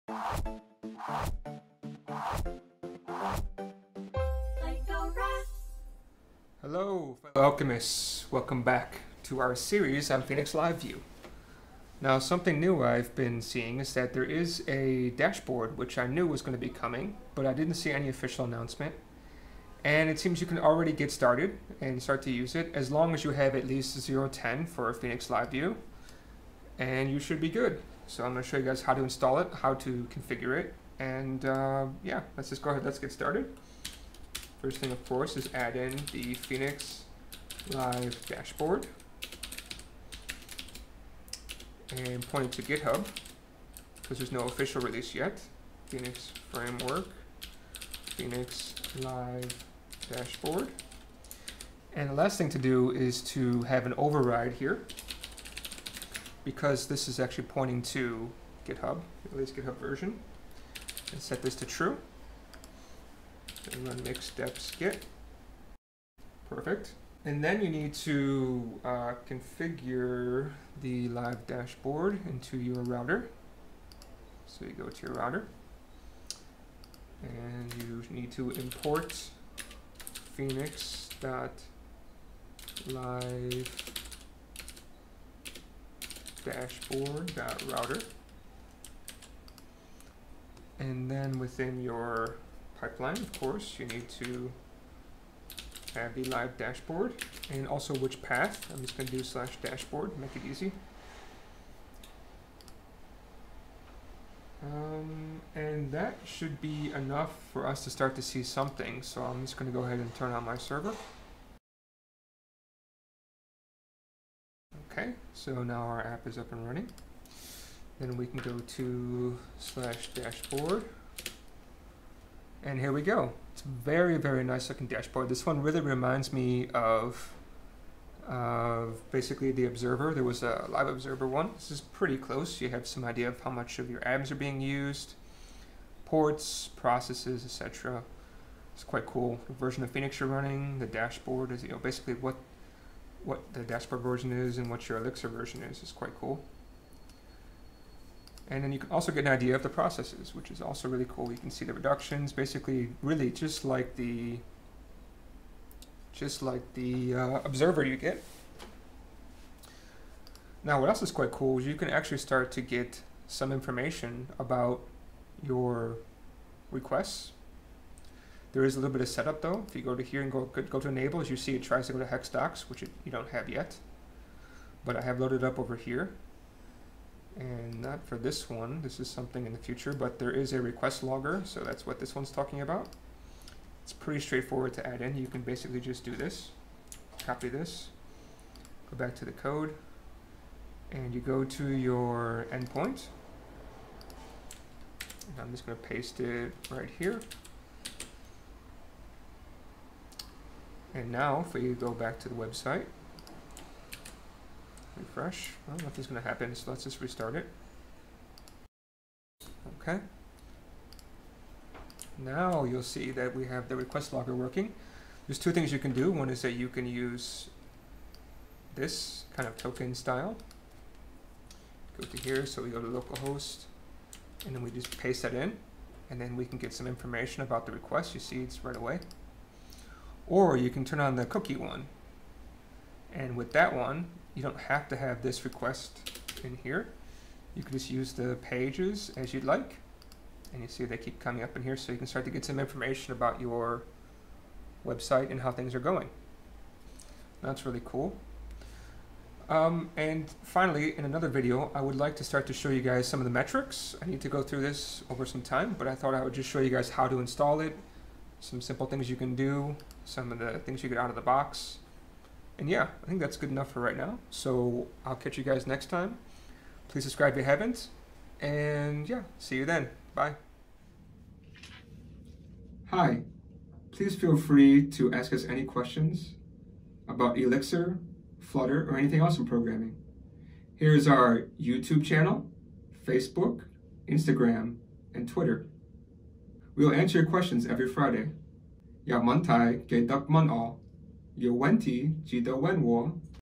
Phoenix LiveView Dashboard
Hello, Fellow Alchemists! We will show you how you can take advantage of the up and coming Phoenix LiveView Dashboard!